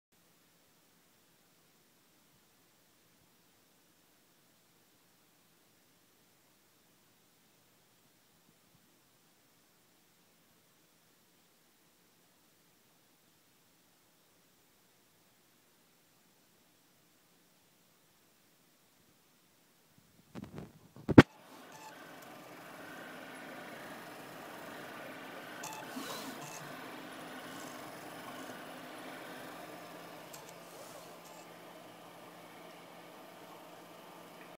Printrbot variant pretending to print
Testing with real gcode and no plastic loaded. There are some minor vibration issues at high speeds, and some beefier rods may be necessary for high-speed printing.